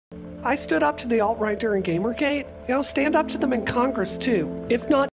I stood up to the alt-right during Gamergate
And I'll stand up to them in congress, too

Source: Brianna Wu for Congress Ad